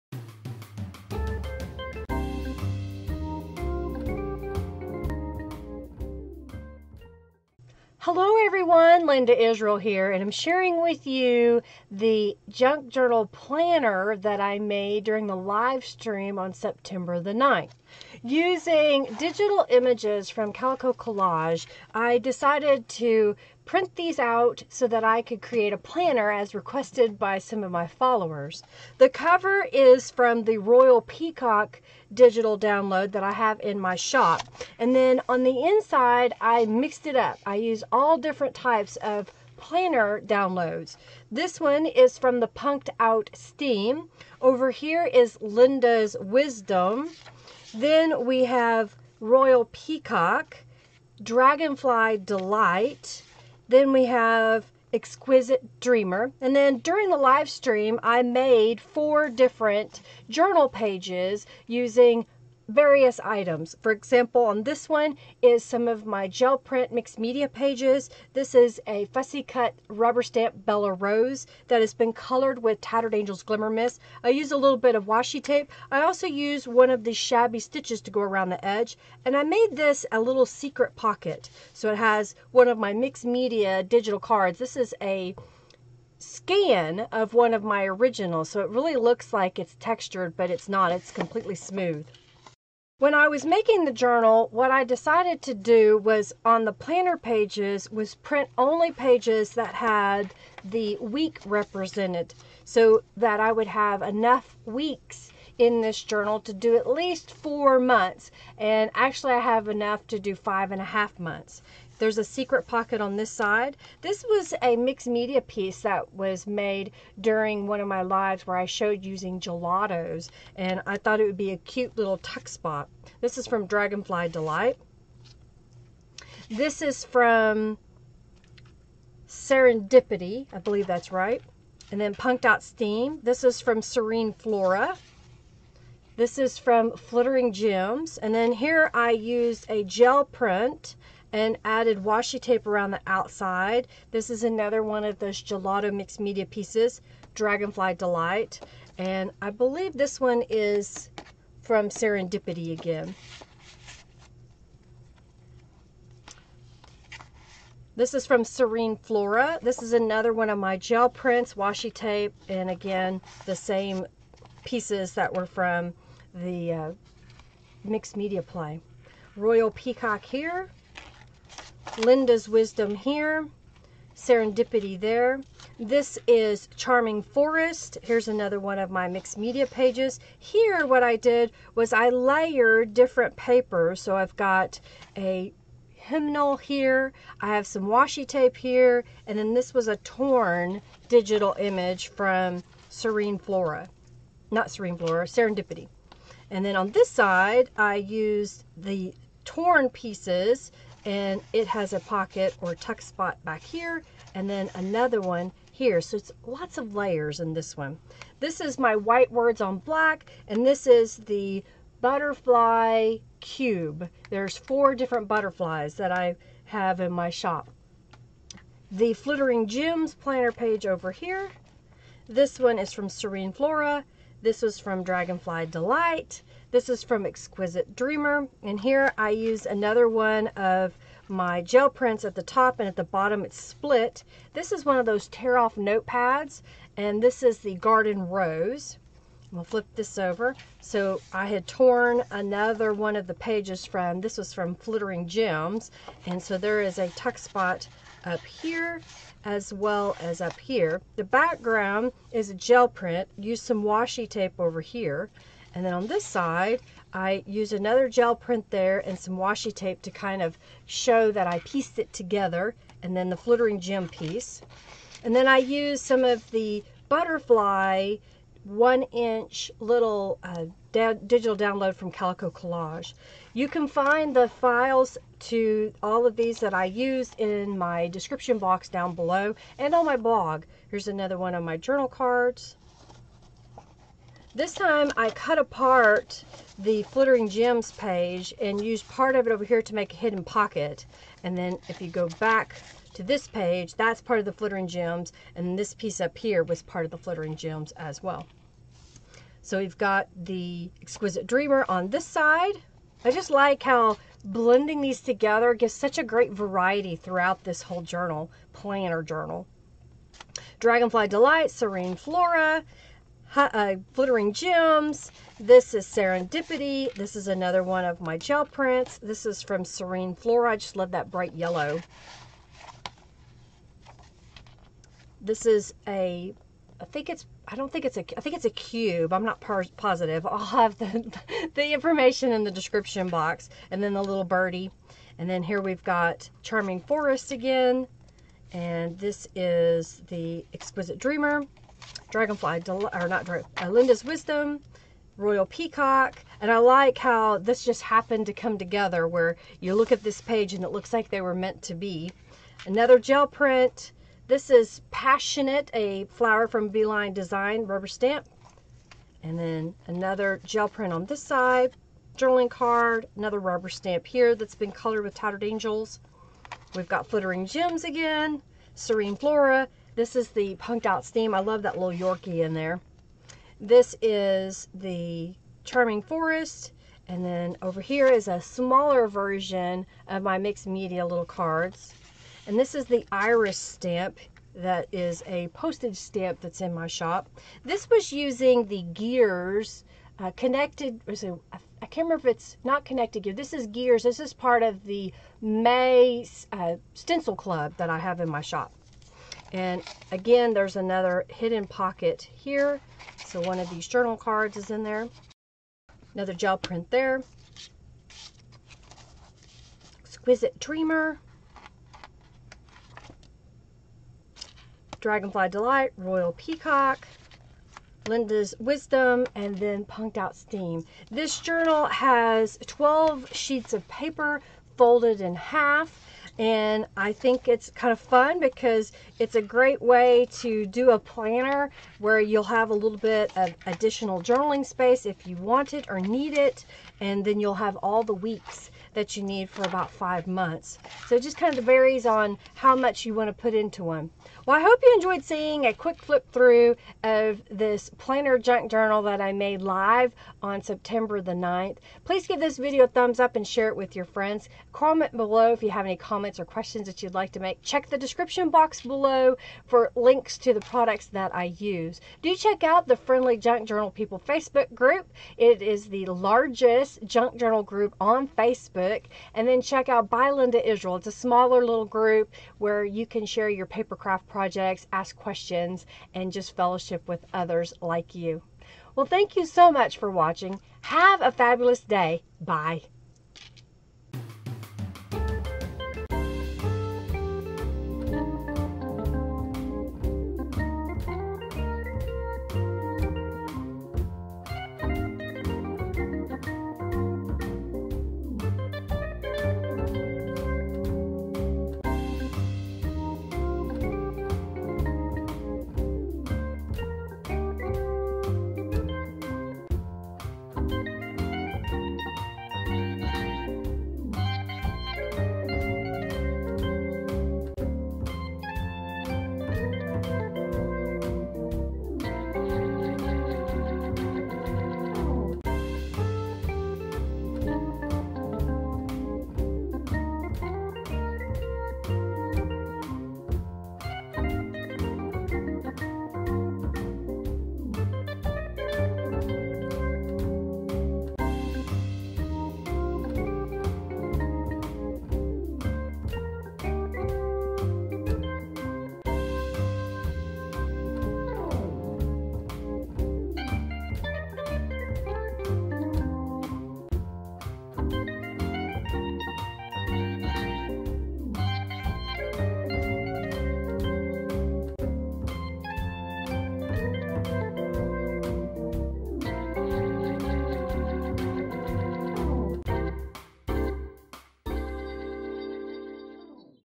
Planner Junk Journal Flip Through
Sharing a Planner Junk Journal Flip Through of the journal made during the live stream on September 9th. Using several digital planner kits by Calico Collage, Rubber Stamps, Stencils and more a journal was made live.

Calico Collage Planner kits
Used pages from the following: One Fine Day, Dragonfly Delight, Flittering Gems, Serene Flora, Exquisit Dreamer, Linda's Wisdom, Serendipity, Charming Forest, Punked Out Steam, Royal Peacock
Distress Ink, Walnut Stain
Blending Tool

Want to write to me? Here is my mailing address:
Linda Israel
818 W Main St
Yukon, OK 73099
USA